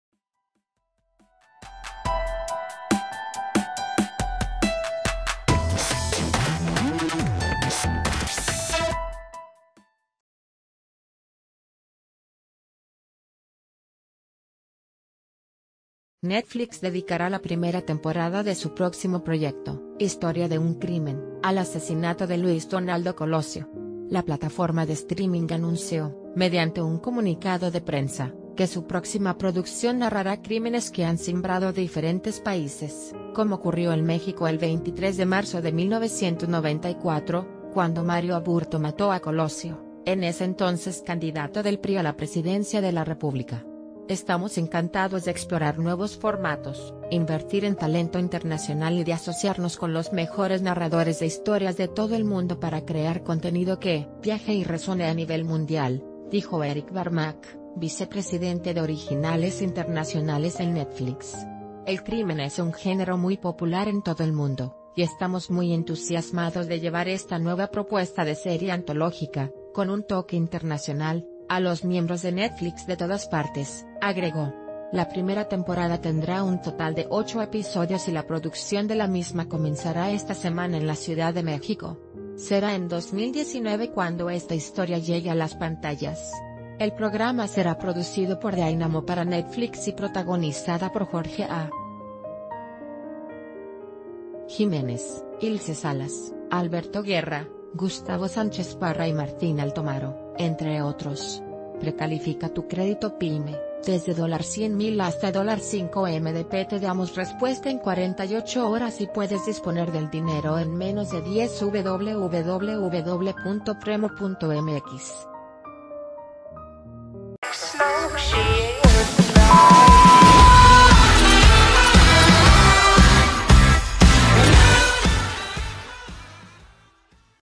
La historia de Luis Donaldo Colosio llegará a Netflix
Netflix dedicará la primera temporada de su próximo proyecto, Historia de un Crimen, al asesinato de Luis Donaldo Colosio.  La plataforma de streaming anunció, mediante un comunicado de prensa, que su próxima producción narrará crímenes que han cimbrado a diferentes países, como ocurrió en México el...